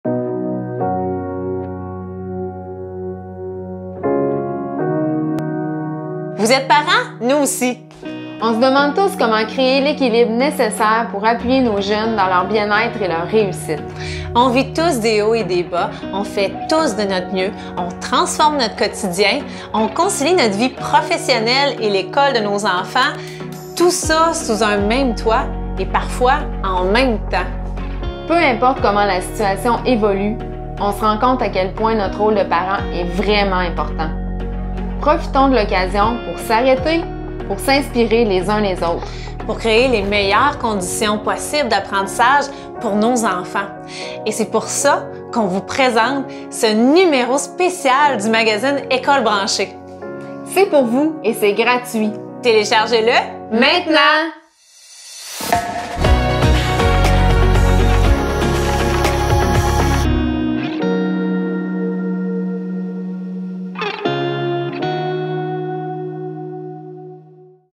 Practical guide for parents - distance learning
How to support your children in their school career without losing your balance?
To help you, we present a real survival guide for parents in times of pandemic!
Don't wait any longer and download it in French or English! It's free!

3 themes that promote well-being are featured in this issue:
- Distance learning;
- Management of emotions and stress;
- Deal with overseeing screen time.

This special issue of École branchée is produced in collaboration with the Fédération des comités de parents du Québec (FCPQ) and the English Parents’ Committee Association of Quebec (EPCAQ). It can be downloaded free of charge thanks to financial support from the Quebec Ministry of Education (MEQ).